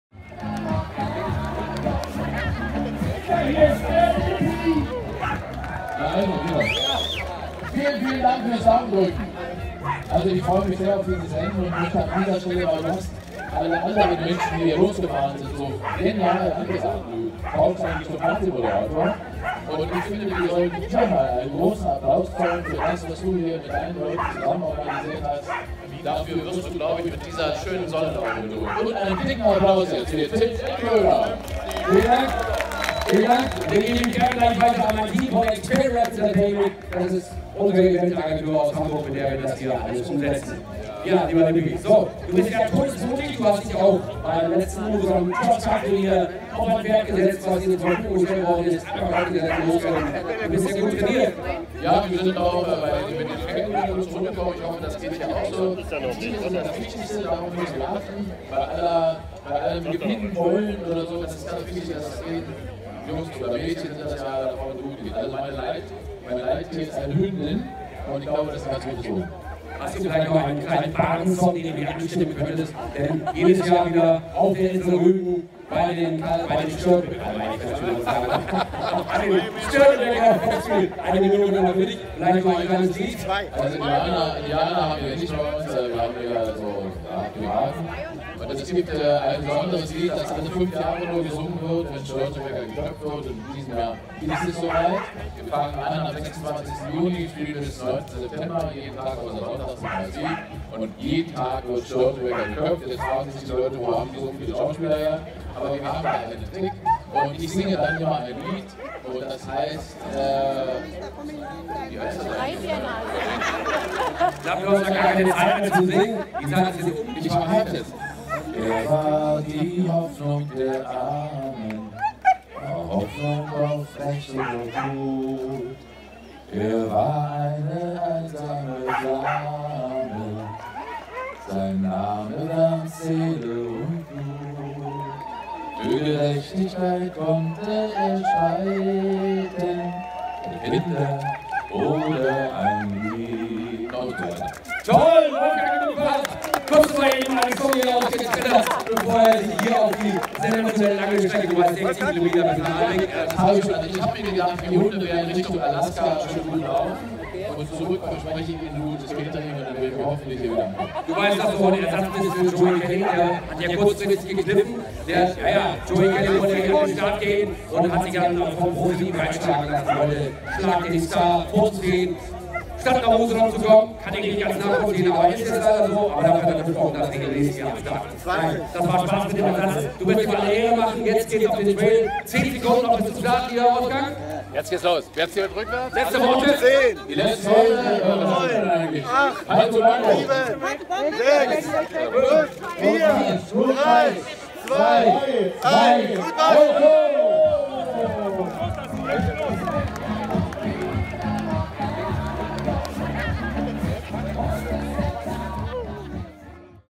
BALTIC LIGHTS 2017
400 Huskys, Rekordspendensumme & Kaiserwetter...
Es hat mir einen Riesenspaß gemacht - Danke an meine Musher - Familie Bobek - Danke an die Hunde, besonders meine Leithündin Nova - Danke an Till Demtröder und den Organisatoren von Baltic Lights - an die Insel Usedom und an das traumhafte Wetter.